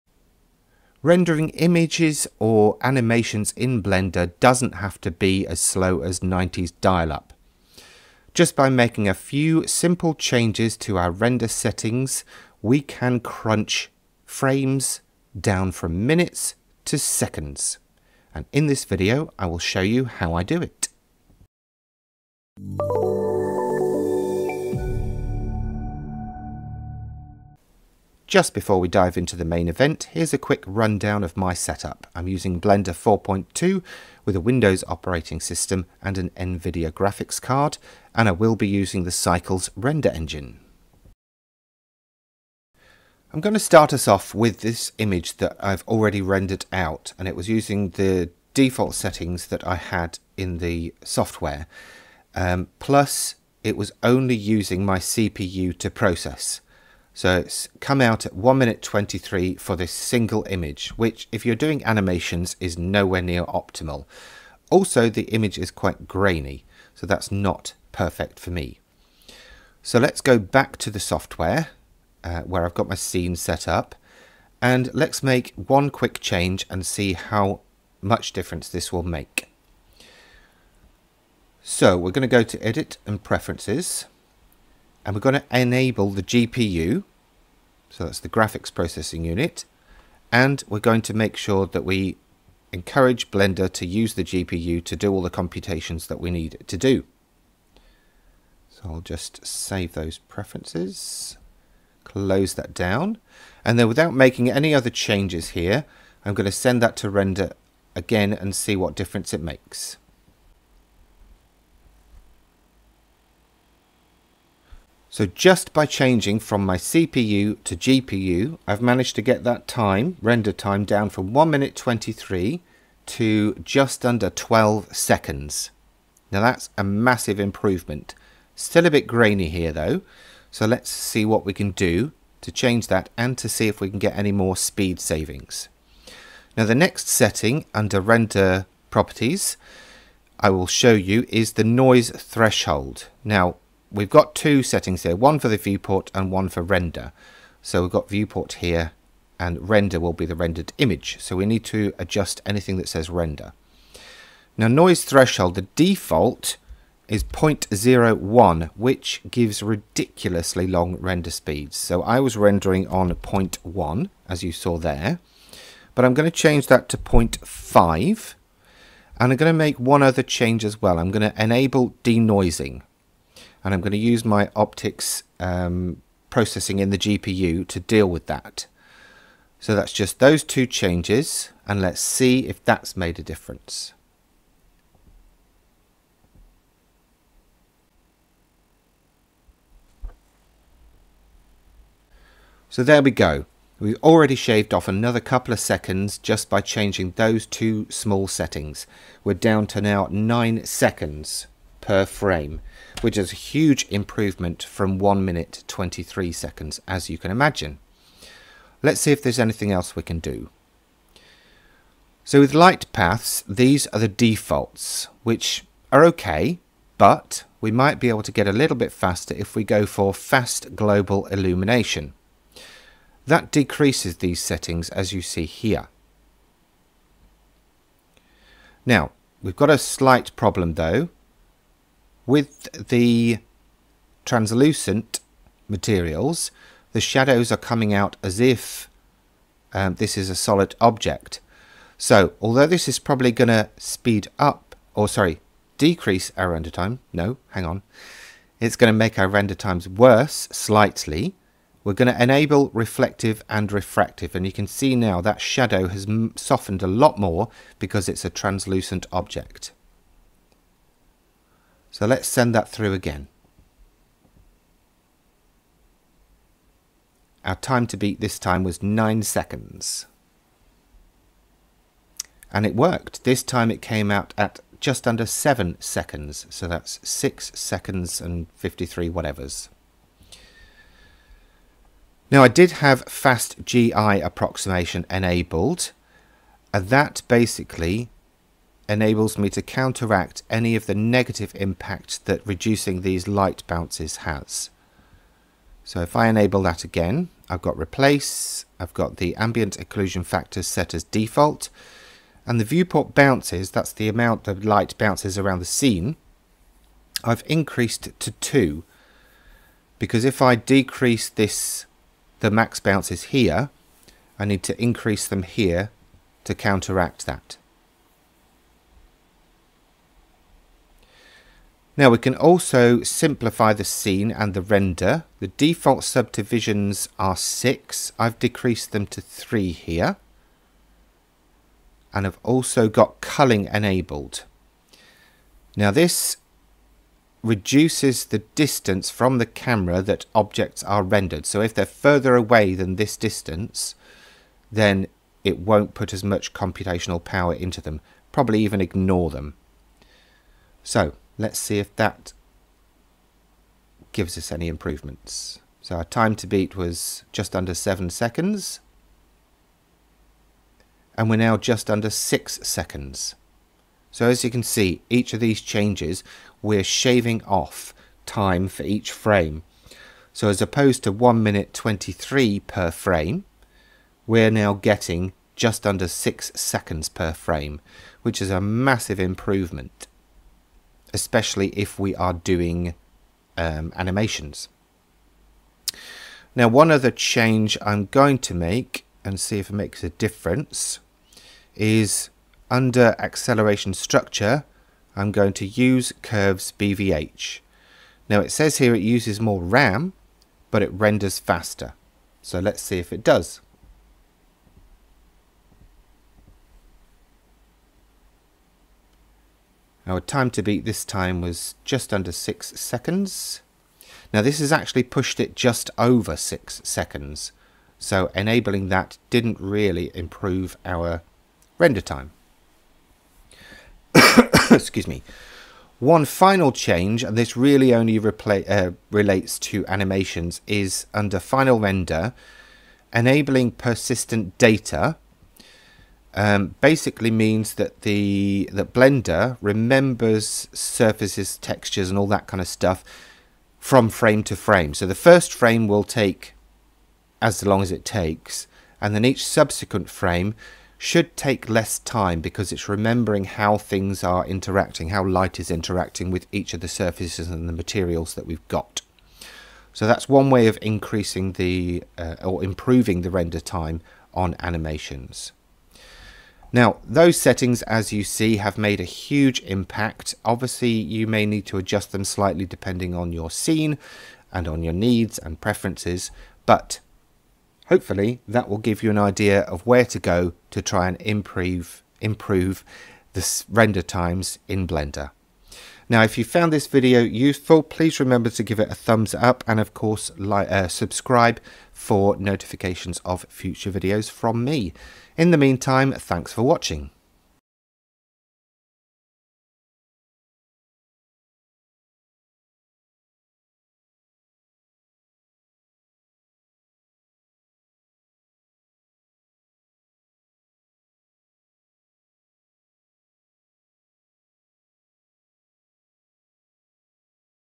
HOW TO SPEED UP RENDERS IN BLENDER
Render times can be a pain but with these simple steps you can speed up your render times to lightening fast!

Chapters
00:00 Introduction
00:22 Titles
00:27 System Set Up
00:42 Benchmark Speed
01:12 Change from CPU to GPU (huge speed upgrade!)
02:15 Change Noise Threshold (another increase in speed!)
03:32 Adjust Light Paths for more speed!
04:42 Fast GI Approximation for a little more speed.
05:20 Simplify (the render) + culling.
07:09 Persistent Data (for Animations)
07:45 Final Thoughts